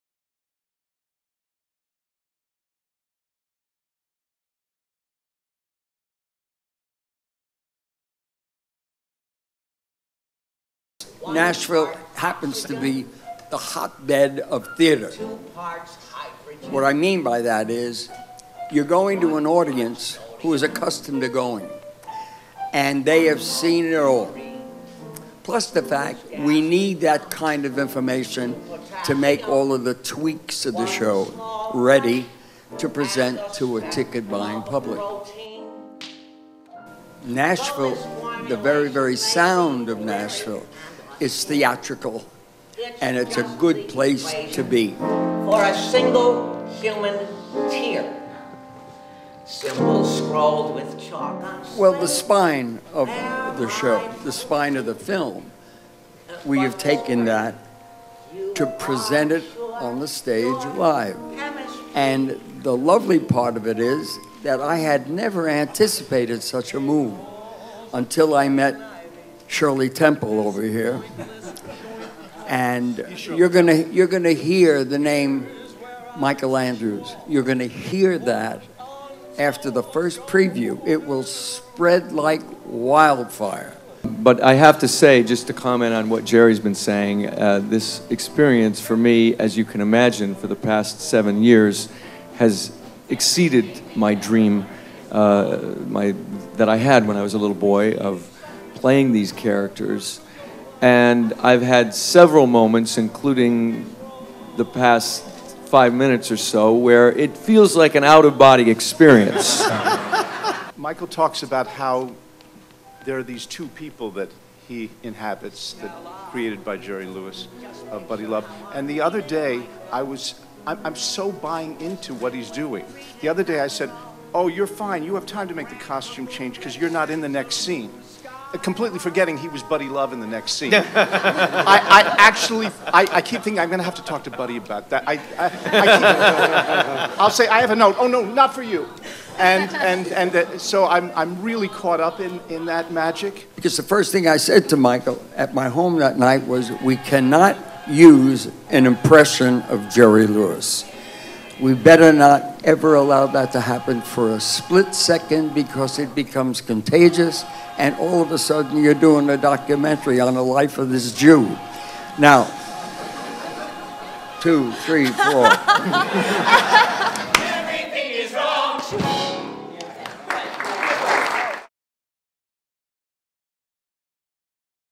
Jerry Lewis and cast on 'Nutty Professor' musical in Nashville
Jerry Lewis directing musical version of role he made famous in the 1960s